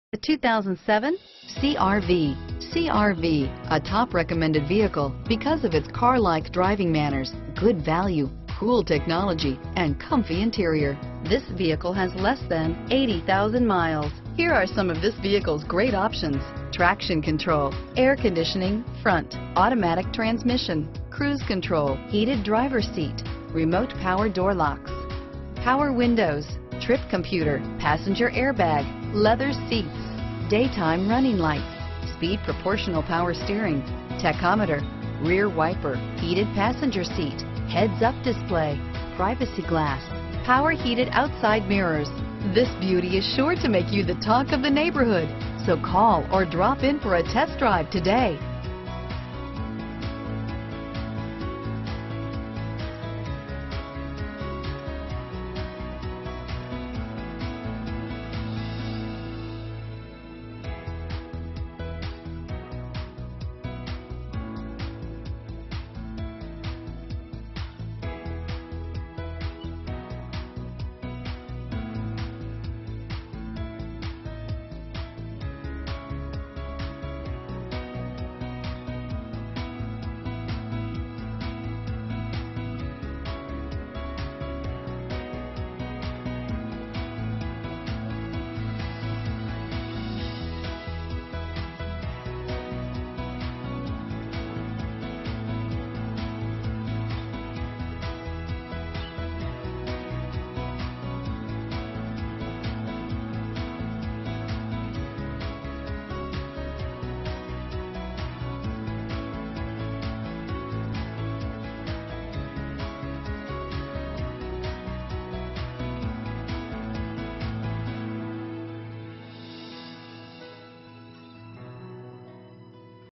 2007 Honda CR-V Used, San Antonio, Austin, Houston, Boerne, Dallas H140323A
2007 Honda CR-V  - Stock#: H140323A - VIN#: JHLRE48777C093709

Gunn Honda
14610 IH-10 W

New In Stock... Gassss saverrrr!!! 28 MPG Hwy! 4 Wheel Drive never get stuck again* Less than 78k Miles! Safety equipment includes: ABS Traction control Curtain airbags Passenger Airbag Daytime running lights...Relax in the comfort of features like: Leather seats Power locks Power windows Sunroof Heated seats...